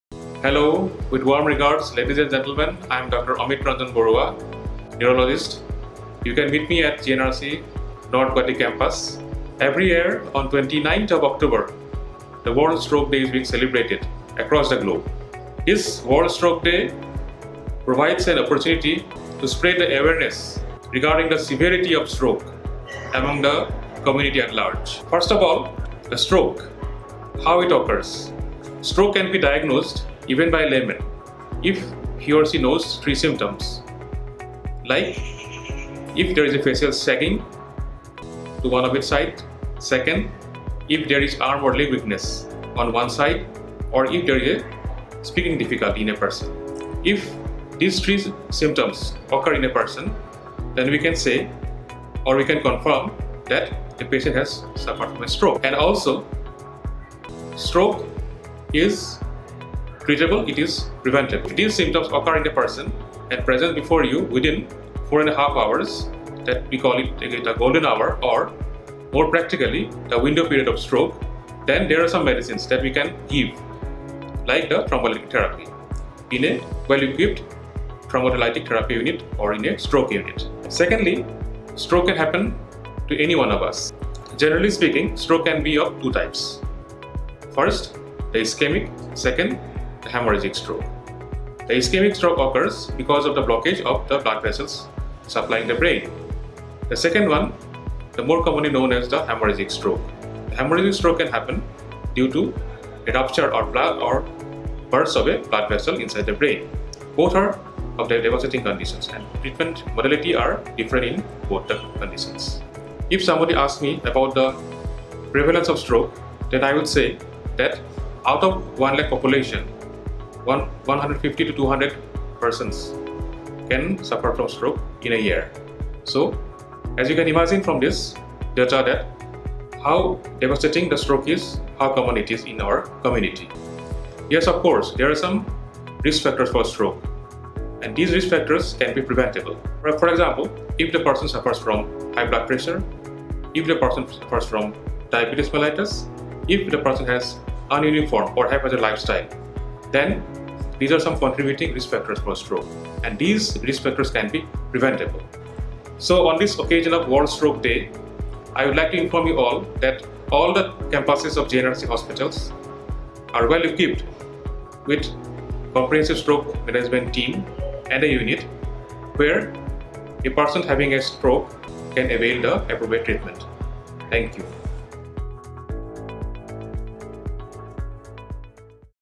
GNRC Hospital | World Stroke Day 2022 | Dr Amit Ranjan Boruah
Dr Amit Ranjan Boruah, Consultant, Neurology dept from GNRC Medical, North Guwahati, speaks about Stroke, its symptoms and treatment in this interview.